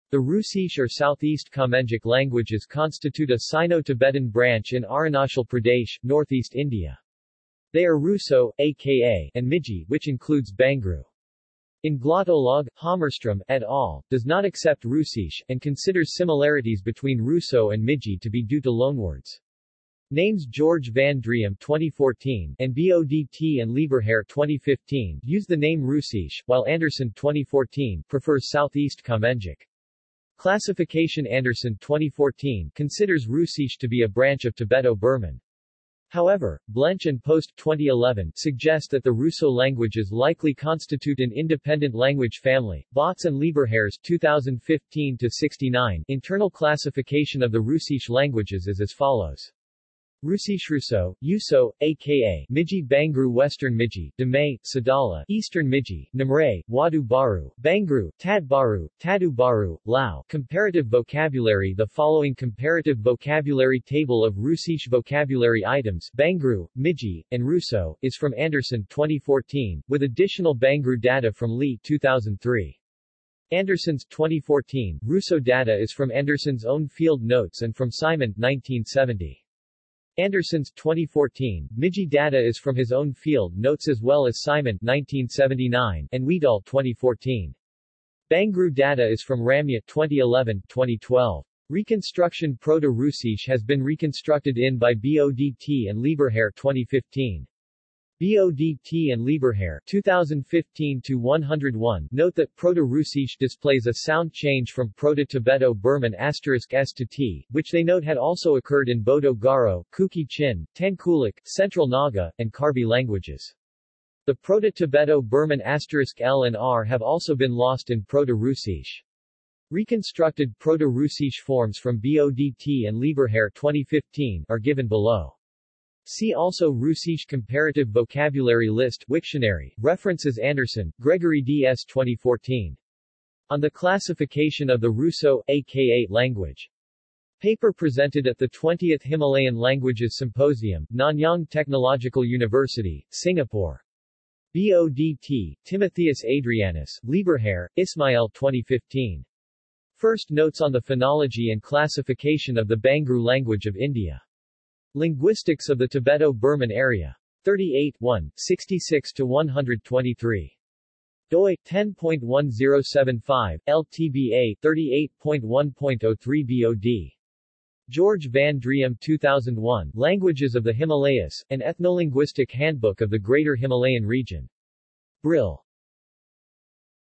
Hrusish languages | Wikipedia audio article
This is an audio version of the Wikipedia Article:
Hrusish languages

SUMMARY
The Hrusish or Southeast Kamengic languages constitute a Sino-Tibetan branch in Arunachal Pradesh, northeast India. They are Hruso (Aka) and Miji (which includes Bangru). In Glottolog, Hammarström, et al. does not accept Hrusish, and considers similarities between Hruso and Miji to be due to loanwords.